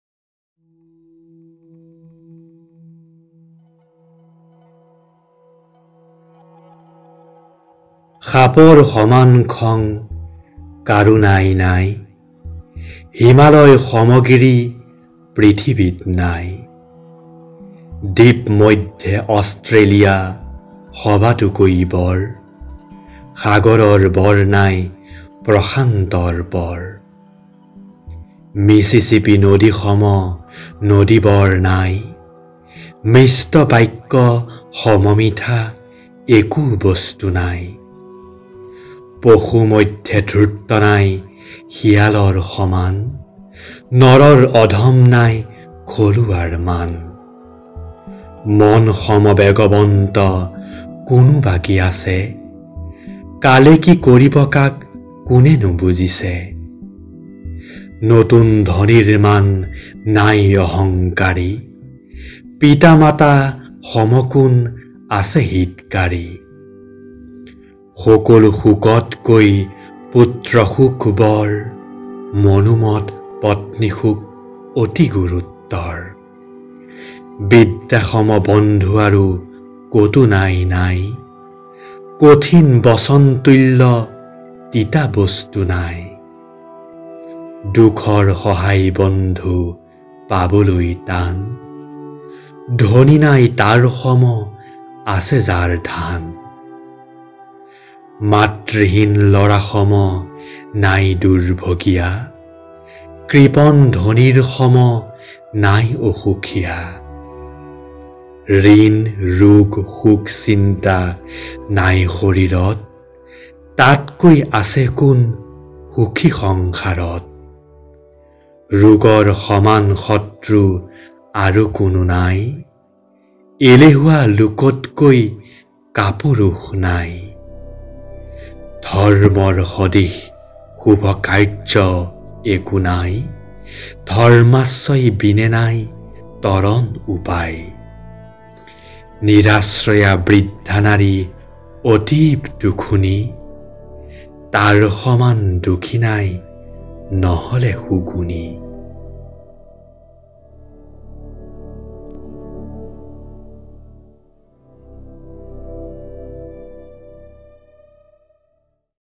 Kaar Saman Ki Nai, Poet Baladev Mahanta, কাৰ সমান কি নাই, কবি বলদেৱ মহন্ত, Assamese Poem Recitation
Poem- Kar xoman Ki nai
Poet- Baladev Mahanta
Recite- Prabal Goswami

কবিতা-কাৰ সমান কি নাই?
কবি-বলদেৱ মহন্ত
পাঠ- প্ৰৱাল গোস্বামী

সাপৰ সমান খং কাৰো নাই নাই
হিমালয় সম গিৰি পৃথিৱীত নাই।

দ্বীপমধ্যে অষ্ট্ৰেলিয়া সবাতোকৈ বৰ,
সাগৰৰ বৰ নাই প্ৰশান্তৰ পৰ।

মিচিচিপি  নদী সম নদী বৰ নাই,
মিষ্ট বাক্য সম মিঠা একো বস্তু নাই।

পশুমধ্যে ধূৰ্ত নাই শিয়ালৰ  সমান,
নৰৰ অধম নাই খলুৱাৰ মান।

মন সম বেগৱন্ত কোনোবা কি আছে?
কালে কি কৰিব কাক কোনেনো বুজিছে?

নতুন ধনীৰ মান নাই অহঙ্কাৰী,
পিতা-মাতা সম কোন আছে হিতকাৰী?

সকলো শোকতকৈ পুত্ৰশোক বৰ,
মনোমত পত্নীশোক অতি গুৰুতৰ।

বিদ্যা সম বন্ধু আৰু ক'তো নাই নাই,
কঠিন বচন তুল্য তিতা বস্তু নাই।

দুখৰ সহায় বন্ধু পাবলৈ টান,
ধনী নাই তাৰ সম, আছে যাৰ ধান।

মাতৃহীন ল'ৰা সম নাই দুৰ্ভগীয়া,
কৃপণ ধনীৰ সম নাই অসুখীয়া।

ঋণ, ৰোগ, শোক, চিন্তা নাই শৰীৰত;
তাতকৈ আছে কোন সুখী সংসাৰত?

ৰোগৰ সমান শত্ৰু আৰু কোনো নাই,
এলেহুৱা লোকতকৈ কাপুৰুষ নাই।

ধৰ্মৰ সদৃশ শুভকাৰ্য্য একো নাই
ধৰ্মাশ্ৰয় বিনে নাই তৰণ উপায়।

নিৰাশ্ৰয়া বৃদ্ধা নাৰী অতীৱ দুখুনী,
তাৰ সমান দুখী নাই নহ'লে সুগুণী।

Assamese Poem Recitation
অসমীয়া কবিতা আবৃত্তি